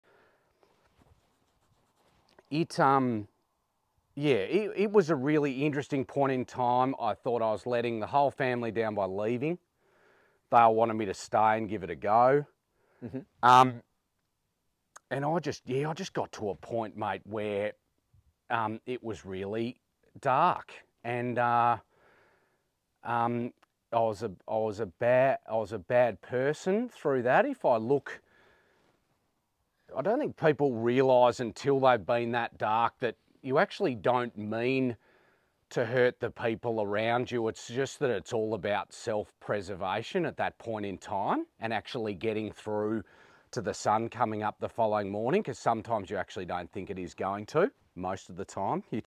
James Knight on Mental Health
A short snippet from the Humans of Agriculture in conversation podcast. James Knight talks about a time when he struggled with mental health upon returning to his wife's family farm.
Check out the full episode under 'Farming, Family, Mental Health and Taking Opportunities'